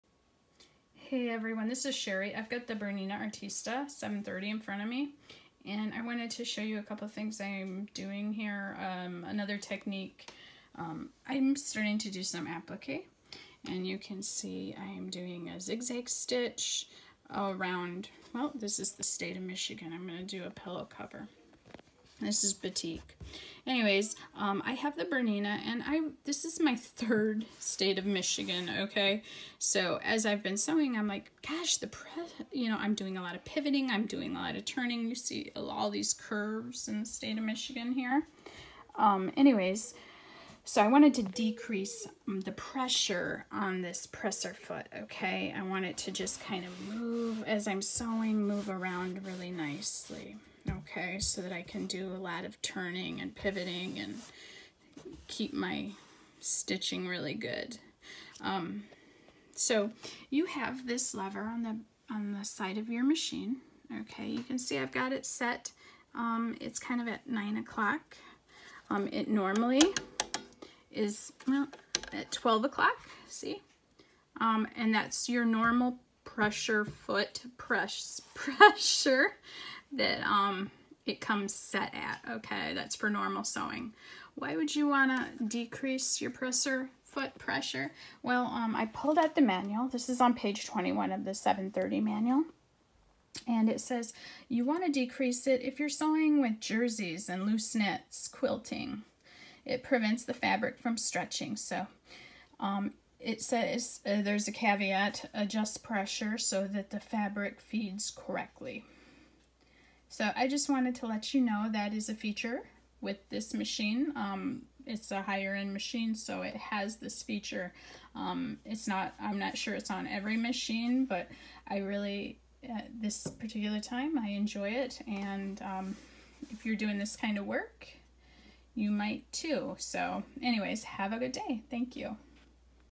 Bernina Artista presser foot pressure adjusted
Have you ever wondered what that lever on the left-hand side of your machine does? This video explains how to change the pressure on your presser foot.